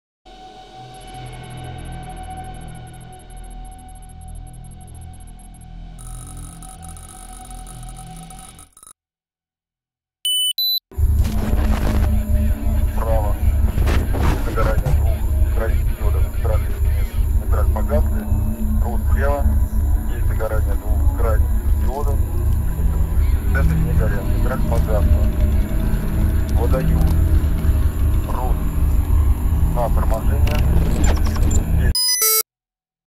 Shrine - first glimpse | Sci Fi | Short Film | Blender
Made this animation back in July. Got this head model from Sketch fab. Initially i had an idea to make an ancient shrine temple with mountains in the back and a river passing through a deep forest where this head will be placed but I dropped that because i didn't know how to model all that. Finally i came up with this where a lost space probe was retrieved and what it recorded in its last moments.